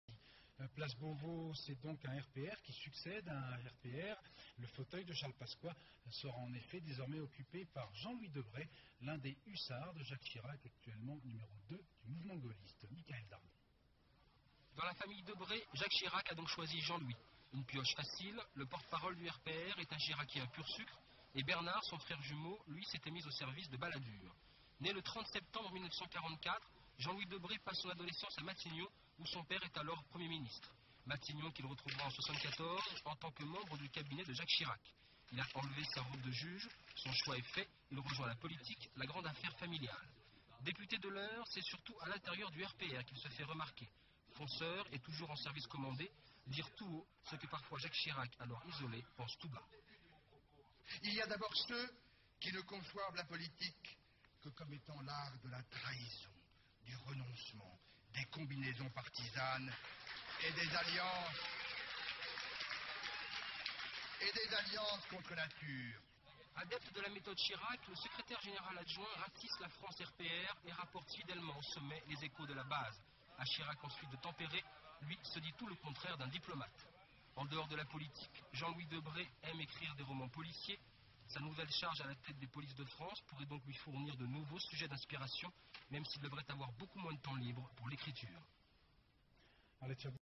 Qui est Jean-Louis Debré ? | Archive INA
18 mai 1995
Portrait de Jean-Louis DEBRE, ancien juge d'instruction, qui a été nommé ministre de l'Intérieur au sein du gouvernement JUPPE. Montage d'images d'archives. politique; archive television; archive tv; ina; inna; Institut National de l'Audiovisuel; french tv Images d'archive INA